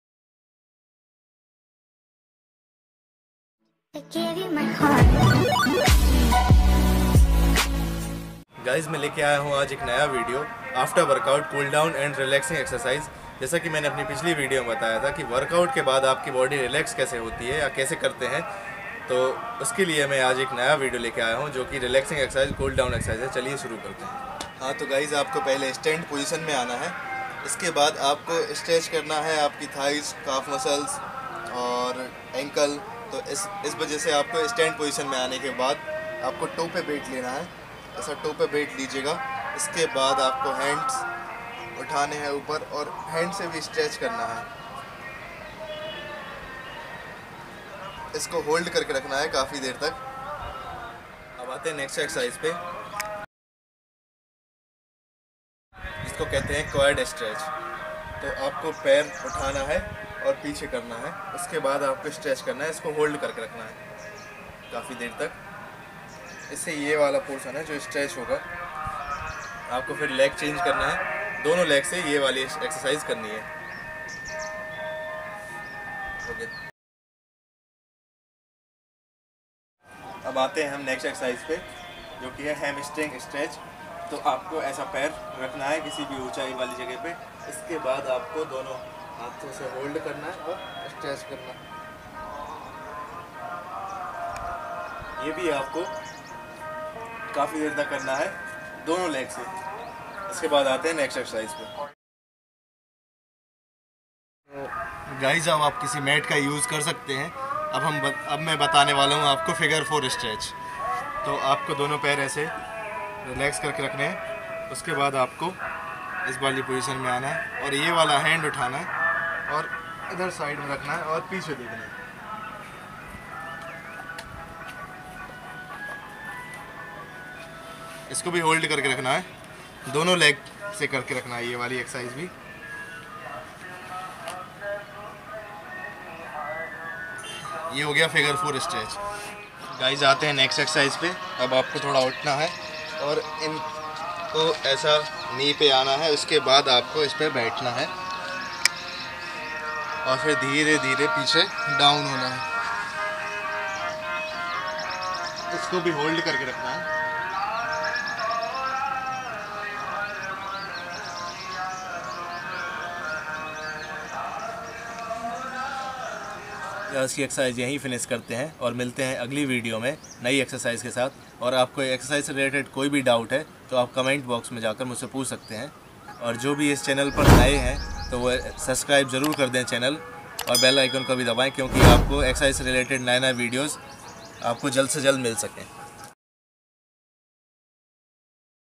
Best Cool Down and Relaxing Exercise | Stretching Exercises
Hey guys aj m aapko batane ja raha hu Cool down exercise kese kare. Or apni body ko after exercise kese relax kare.

XXXXXXXXXXXXXXXXXXXXXXXXXXXXXXXX

Intro credit:-

XXXXXXXXXXXXXXXXXXXXXXXXXXXXXXXX